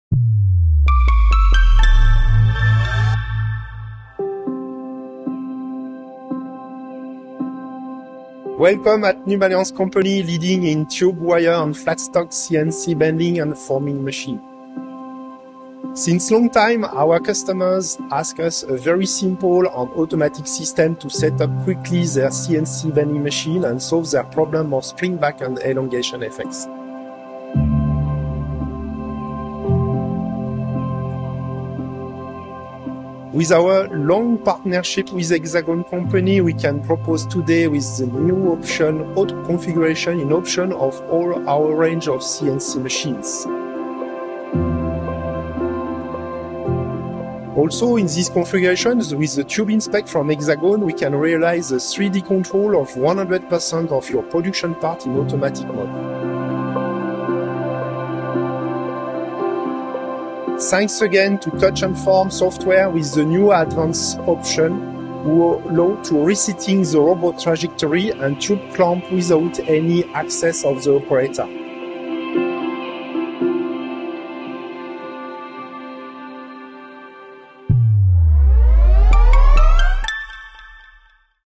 Pionieri della piegatura dei tubi: Intervista a Numalliance
Abbiamo visitato Numalliance, azienda leader nella produzione di macchine di piegatura, per parlare con Hervé Lasnier, Regional Sales Director, del loro ultimo progetto: un sistema di verifica dei tubi completamente automatico basato sulle funzioni uniche del nostro sistema TubeInspect HRC.